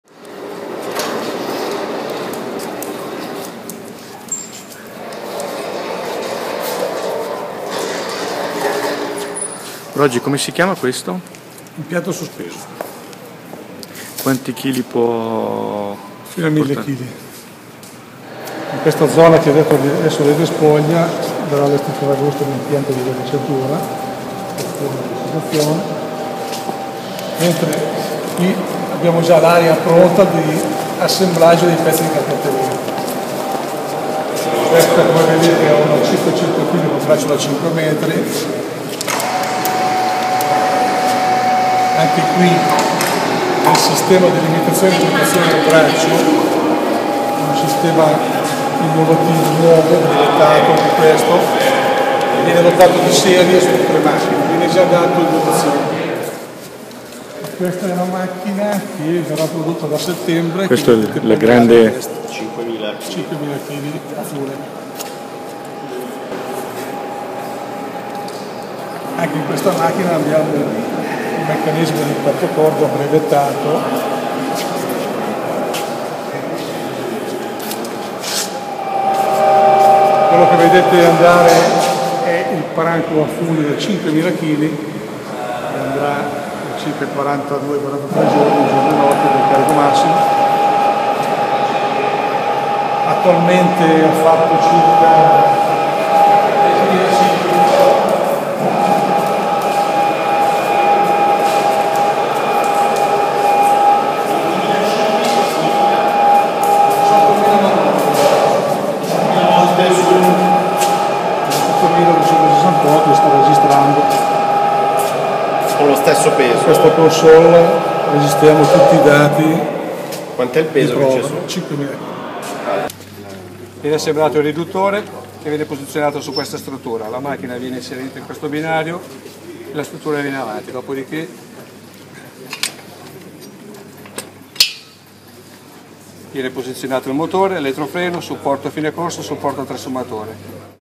Porte aperte alla VHT
La multinazionale tascabile di Bodio Lomnago ha aperto le proprie porte a clienti, fornitori, giornalisti e curiosi. L'amministratore delegato Libero Donati ha invitato i colleghi imprenditori a copiare il loro modello.